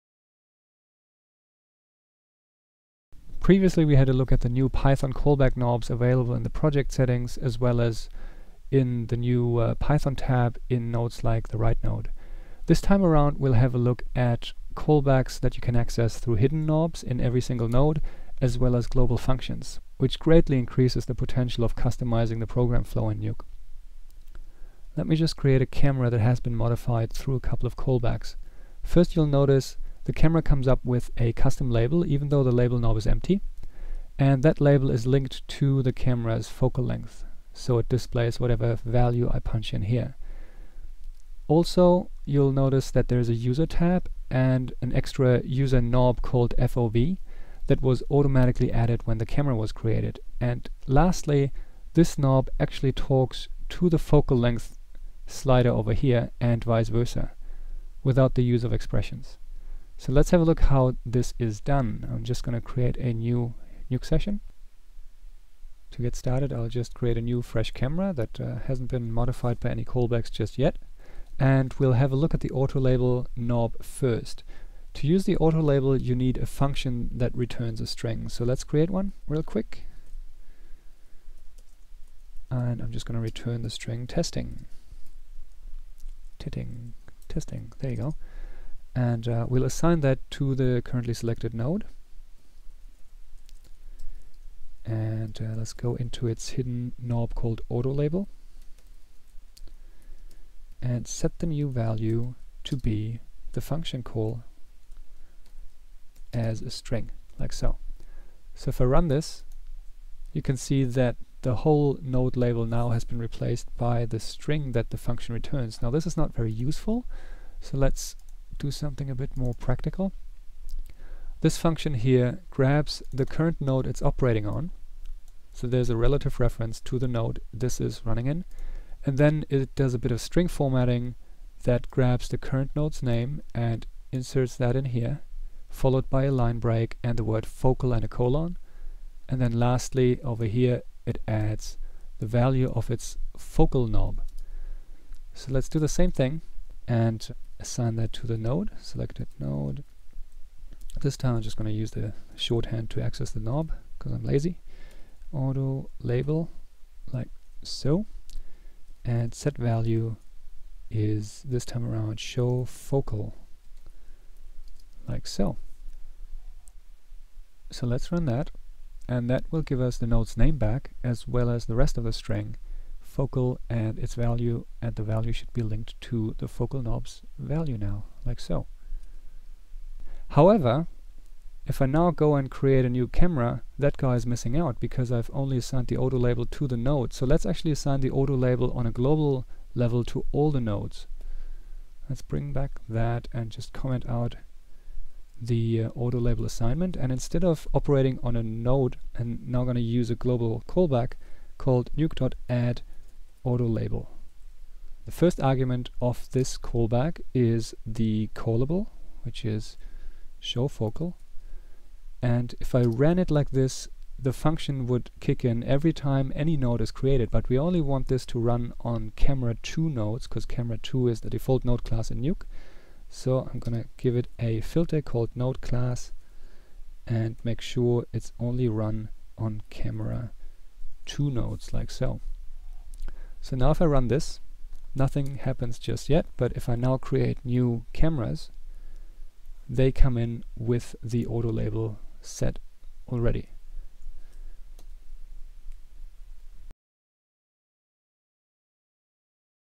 Nuke | Python For Artists: Callback Functions (Part 1)
Frank Rueter explains callback functions.